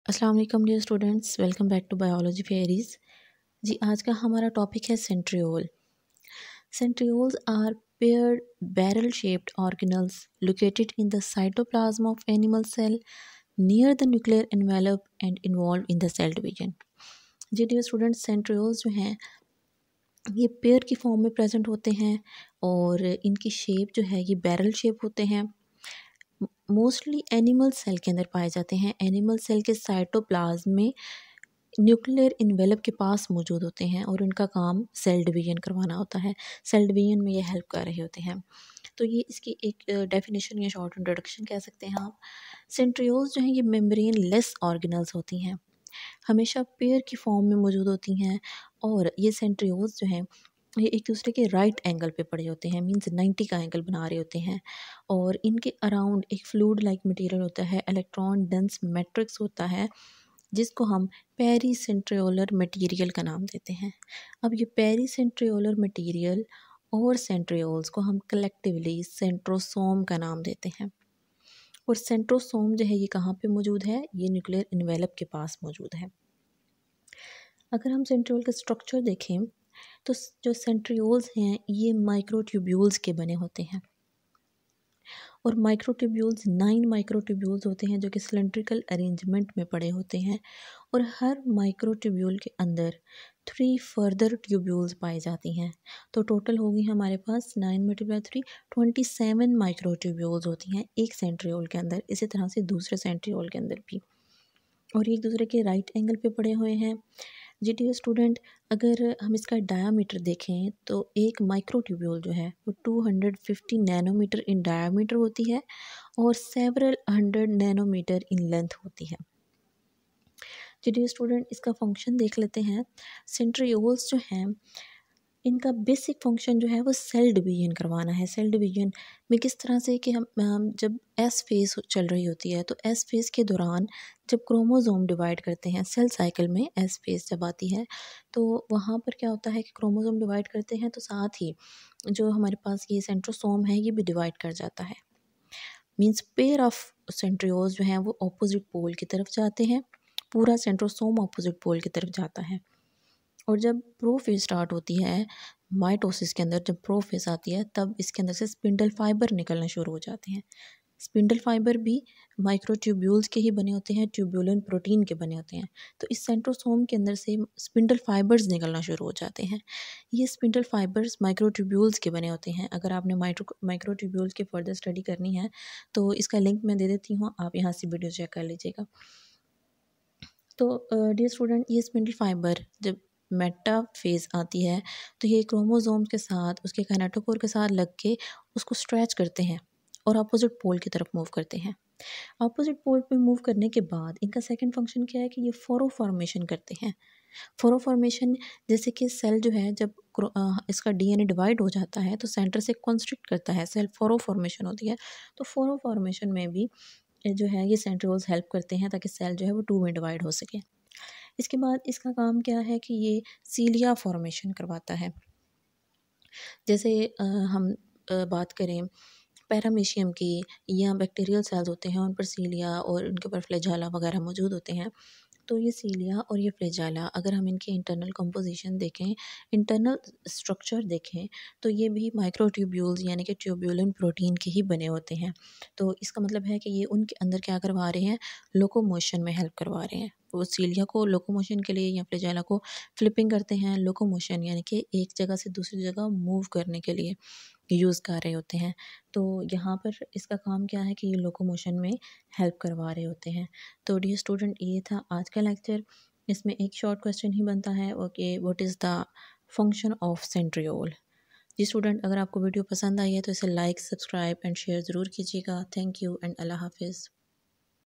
4.14 Centrioles | Structure and function | Cilia & Flagella | F.Sc class 11 biology chapter 4
Centrioles
Structure and function of centrioles
Cilia and flagella
Size and structure of microtubules
Fsc class 11 chapter 4 bioligy
Biology fairies
~Cytoskeleton and its types